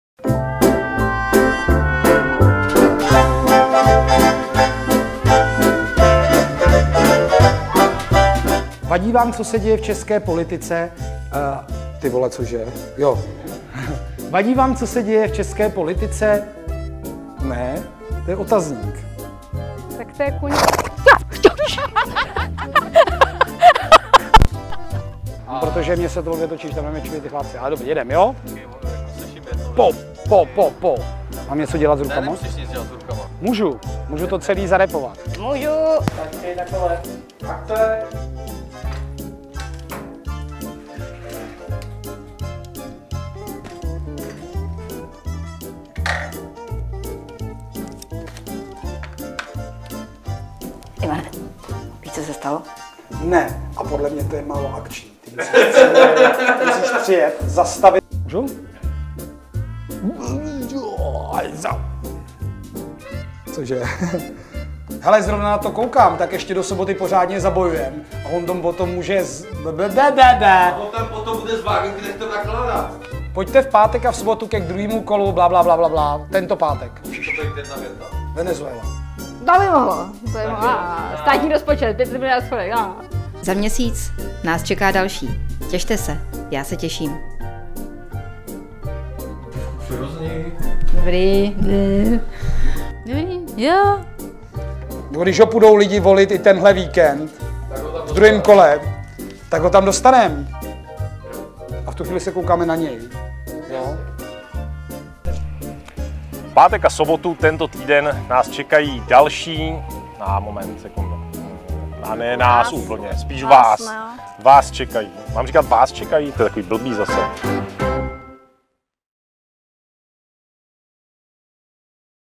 Piráti: nepovedené záběry 2020
Letos jsme pro vás natočili hromadu videí. Někdy to byl porod, ale zasmáli jsme se :) Zasmějte se s námi! Přejeme krásný rok 2021!